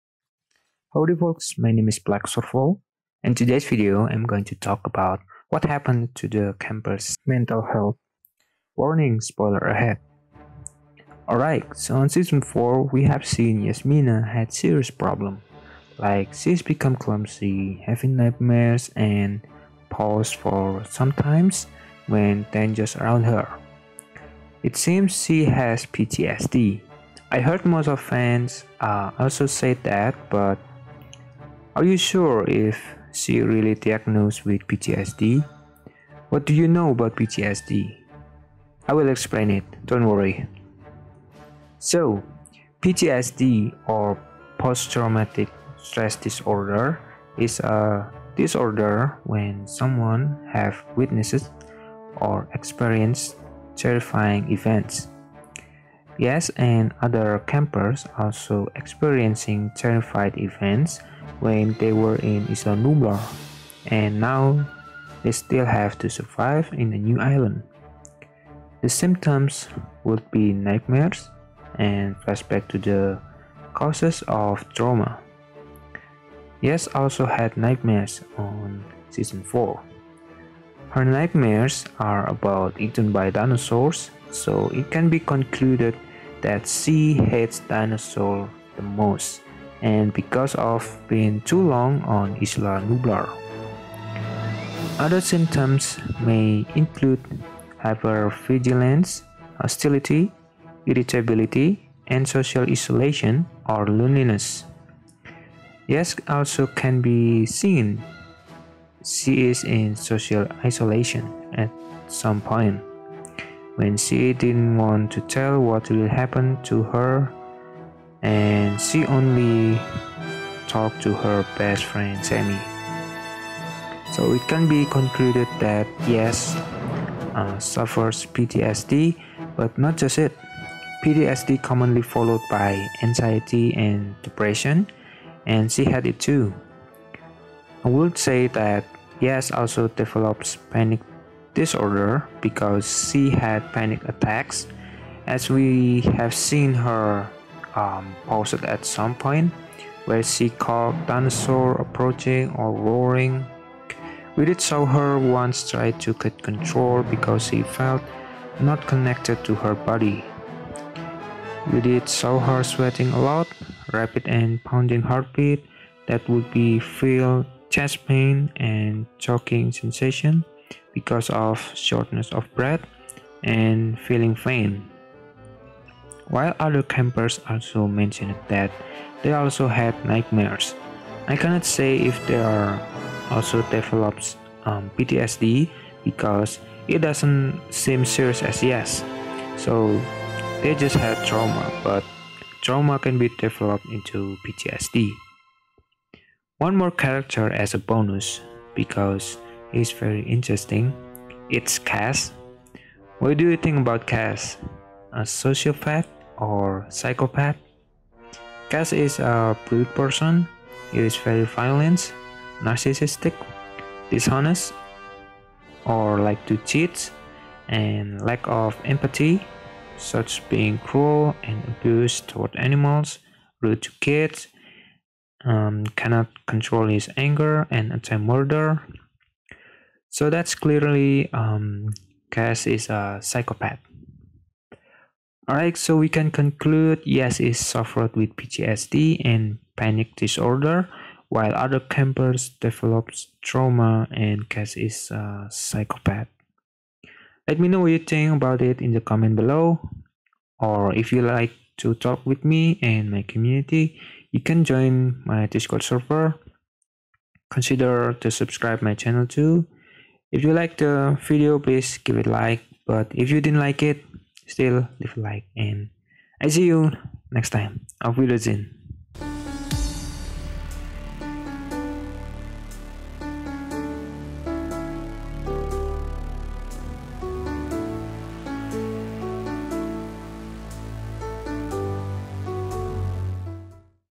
WHY IS YAZ HAVING NIGHTMARES? MENTAL HEALTH BREAKDOWN - Camp Cretaceous Season 4 | Analysis
What happened to Yaz and her friends? Why are they having nightmares? Today's video we're analysis their mental health, how surviving from dinosaur could affects their mental health.
Yaz had Nightmares | Mental Health | PTSD | Panic Attack | Panic Disorder | Trauma | Psychopath | Anxious | Depression | Disorder

About this film:
Six teens invited to attend a state-of-the-art adventure camp on Isla Nublar must band together to survive when the dinosaurs break out of captivity.